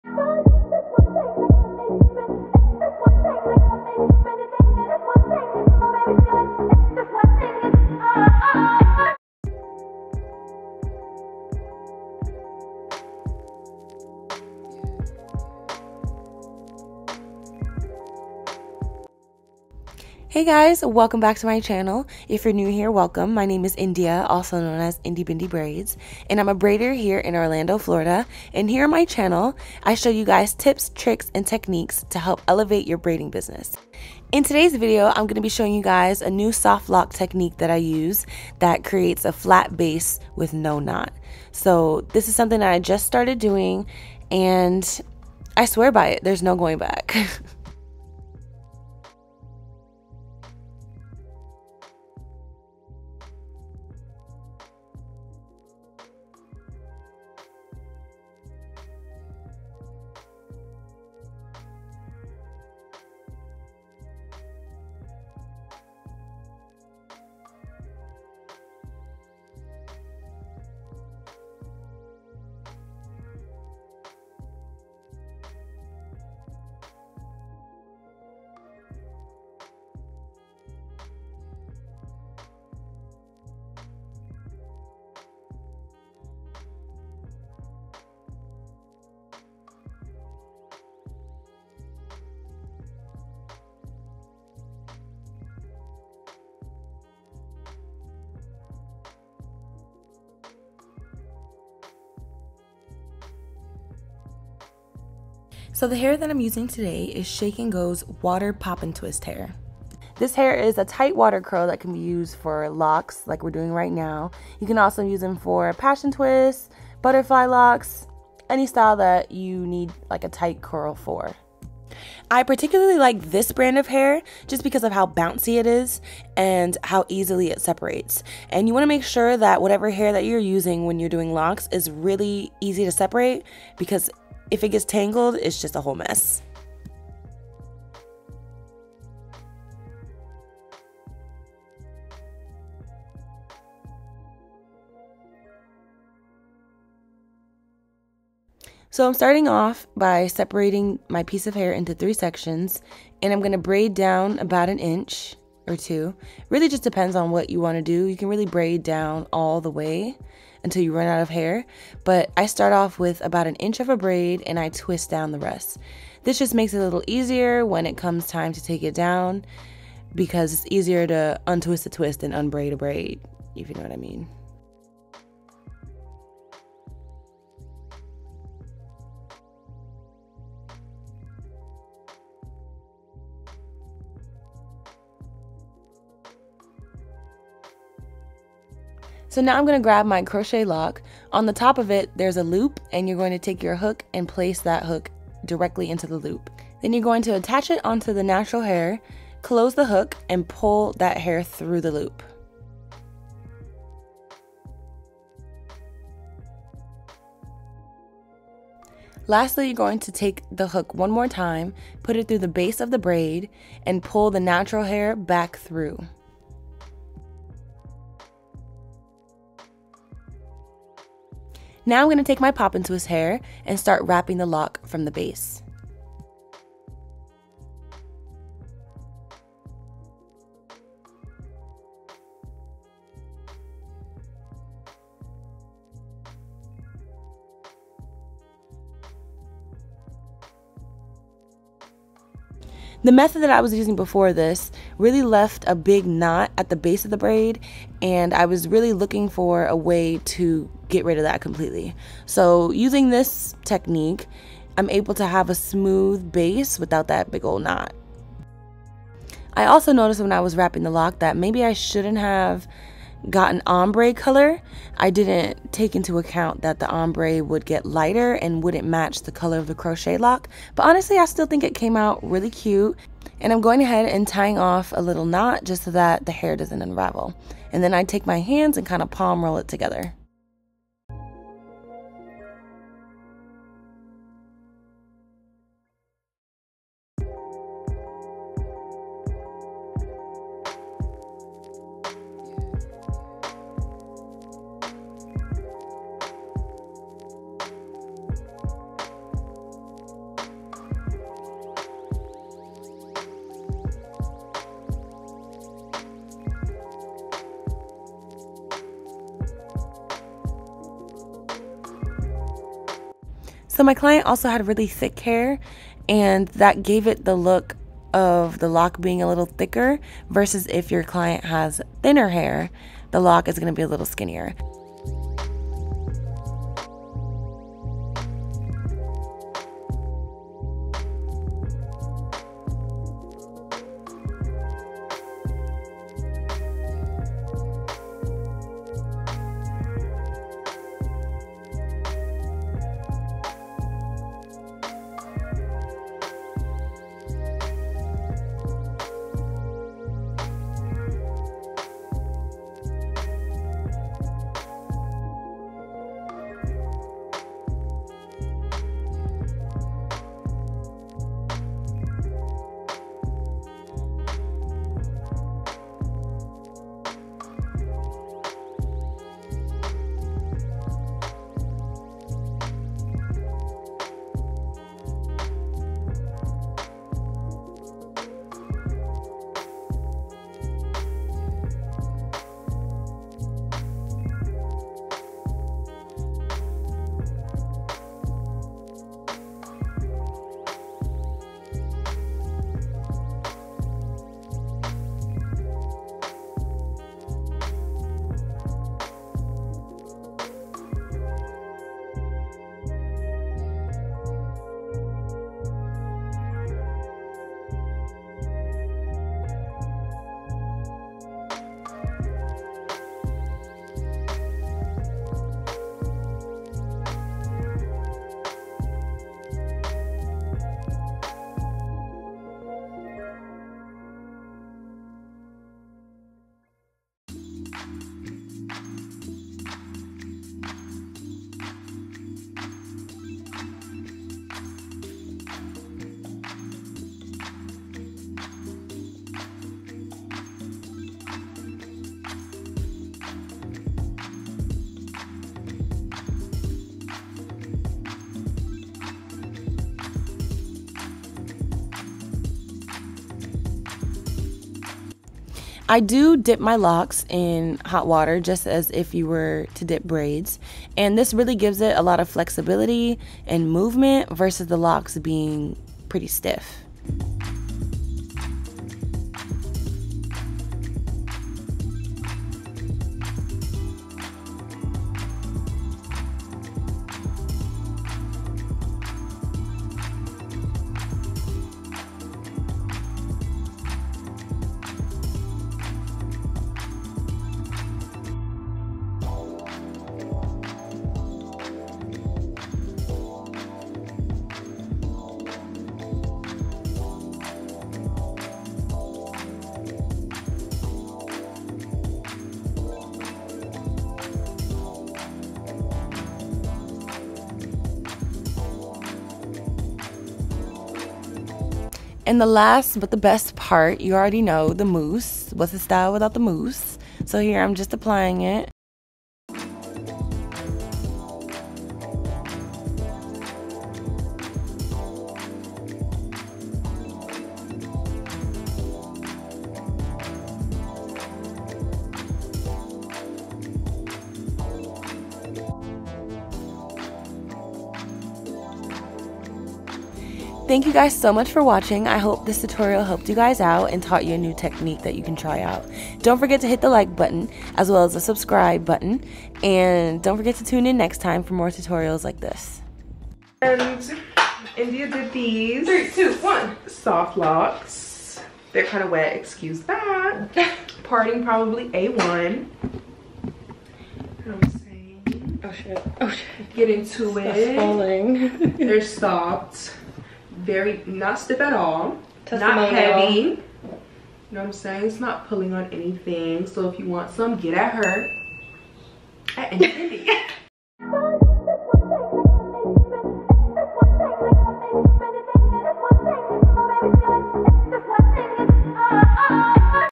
SOFT LOC TUTORIAL | New Technique For Flat Install | NO Knot
Looking for a NO KNOT method of soft locs?? Look no further...

♡ products:
Style: Softie Locs 24"
Length: Waist-Length
Time: 7 hrs
Color: T27
Products used: 3 packs Shake n Go 3x Poppin Twist, 3 bundles of Freetress Crochet Loc
Shine n' Jam (red one)

Q & A
☼ where are you located? orlando, florida
☼ where are you from? florida
☼ how long have you been braiding? 5 years

Q & A
☼ where are you from? pembroke pines, fl